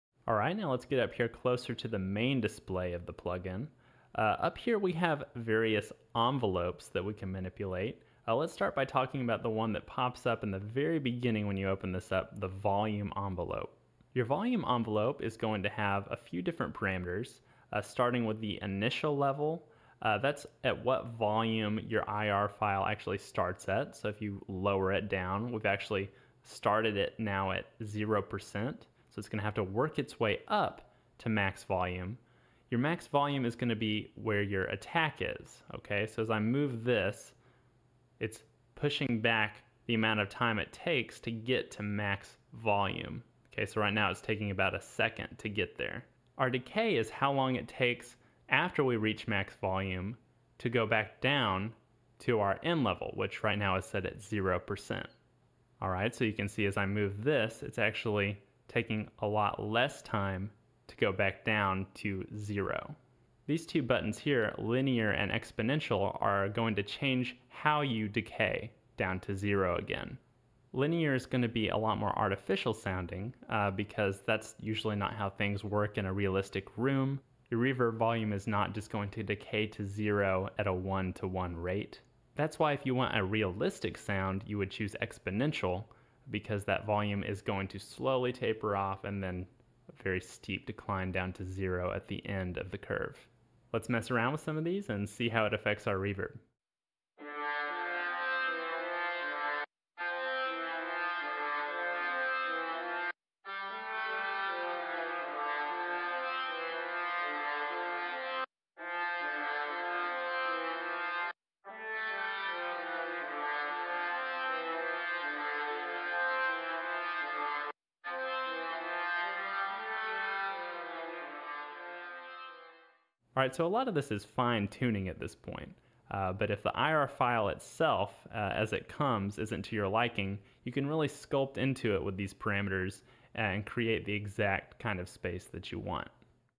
Space Designer Reverb Tutorial Series (Logic Pro X) | Volume Envelope: Automating Your Reverb Volume
What is Space Designer? What is a convolution reverb? What is an impulse response file?

This tutorial is meant to provide you with an in-depth explanation of the different parts of the space designer plugin, so that you can approach your own tracks with the confidence of knowing when and how to apply it.

We will be using an instrumental composed of three instruments, each utilizing a differently sized reverb, to discuss the various parameters of this plugin in Logic Pro X. Here are the parts that we will be discussing in this video:

- Volume Envelope
- Init Level (Initial Level)
- Attack
- Decay
- LIN (Linear) & EXP (Exponential)
- End Level